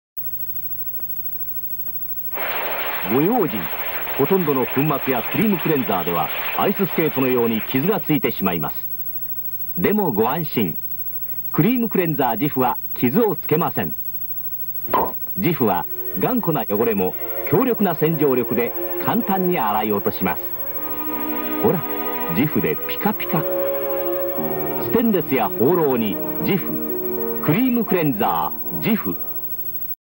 【懐かしCM】クリームクレンザージフ 30秒 1985年
の30秒版を見つけたのでアップしてみます。
後半のBGMの曲名はわかりませんが、捜索の手がかりになれば。

【録画データ】1985年6月　NST新潟総合テレビ